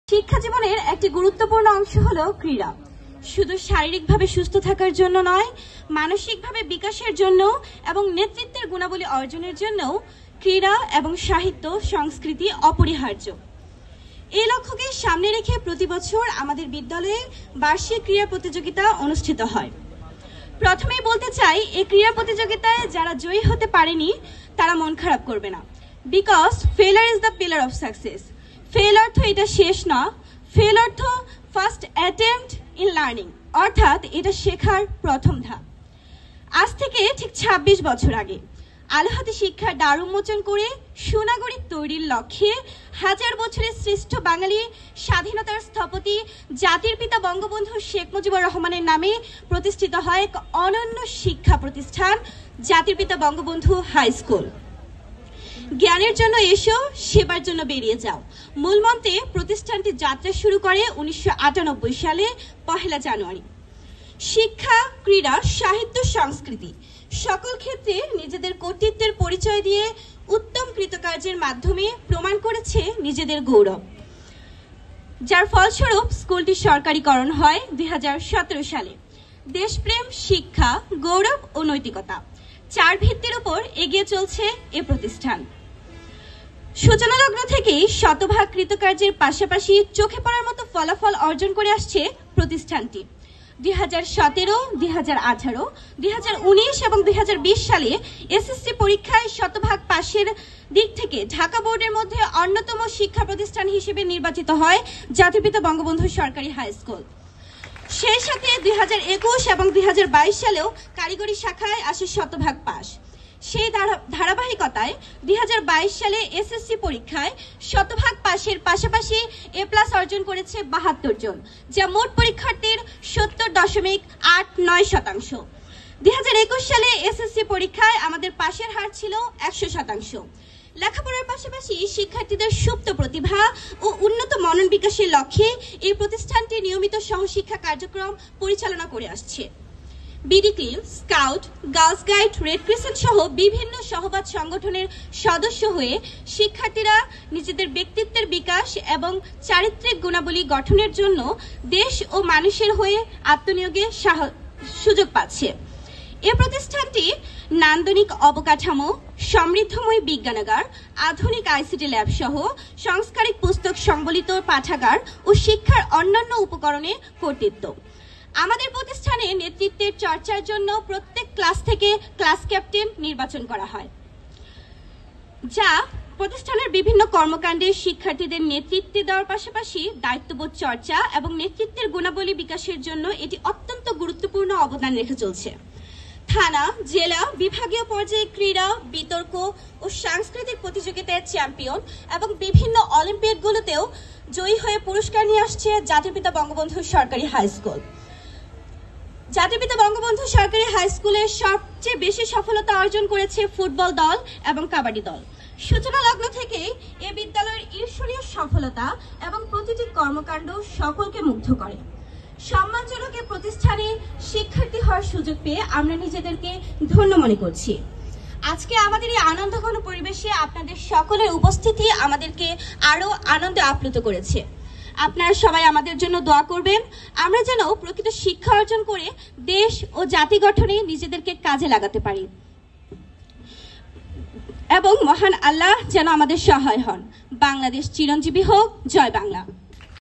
শিক্ষার্থীর বক্তব্য। বার্ষিক ক্রীড়া প্রতিযোগীতা, সাংস্কৃতিক ও পুরষ্কার বিতরণী অনুষ্ঠান-২০২৪। JPBBGHS
বার্ষিক ক্রীড়া প্রতিযোগীতা, সাংস্কৃতিক ও পুরষ্কার বিতরণী অনুষ্ঠান-২০২৪
প্রধান অতিথি- জনাব মোঃ সেলিম আজাদ, চেয়ারম্যান, উপজেলা পরিষদ,
                        কালিয়াকৈর, গাজীপুর।
বিশেষ অতিথি- জনাব মোঃ মনোয়ার শাহীন, ভাইস-চেয়ারম্যান, উপজেলা পরিষদ,
                         কালিয়াকৈর, গাজীপুর।
সভাপতি-         আনন্দ কুমার দাস, প্রধান শিক্ষক, জাতির পিতা বঙ্গবন্ধু সরকারি হাই
                        স্কুল, কালিয়াকৈর, গাজীপুর।

উক্ত অনুষ্ঠানে শিক্ষার্থীদের পক্ষ থেকে সংক্ষিপ্ত বক্তব্য পেশ করে রুবাইয়া তাবাসসুম প্রমি, দশম শ্রেণি।